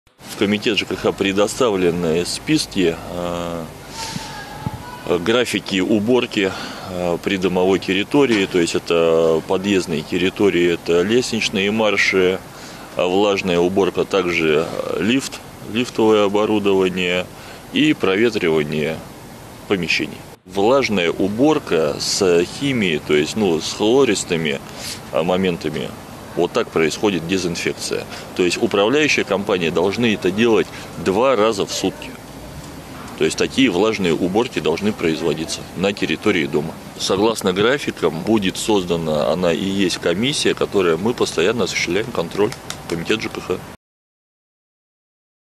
Наш дом – наше здоровье!
За борьбу с коронавирусом взялись управляющие компании. Пока не все, но некоторые стали более тщательно убирать подъезды и лифтовые кабины. Дезинфекция должна проводится в каждом подъезде. Коммунальщики должны вывесить графики уборки, а после её проведения отчитаться перед жителями. В случае несоблюдения управляющими компаниями установленного графика жители могут пожаловаться в комитет ЖКХ или Госжилинспекцию.
Александр Невский